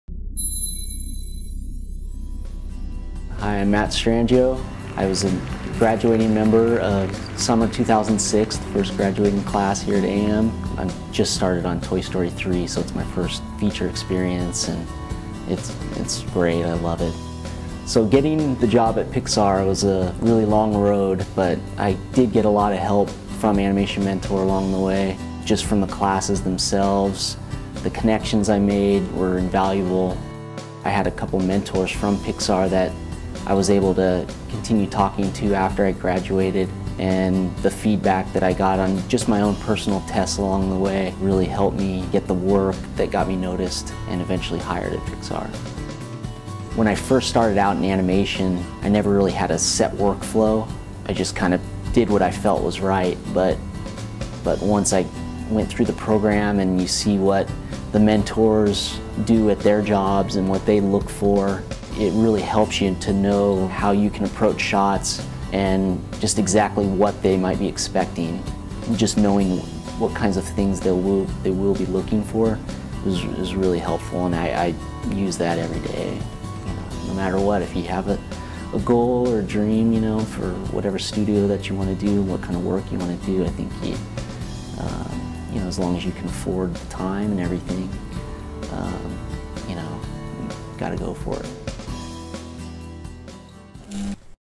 Matt Strangio - Pixar Animator and Animation Mentor Graduate
Interview with Matt Strangio, a member of Animation Mentor's first graduating class who now works as an animator at Pixar Animation Studios.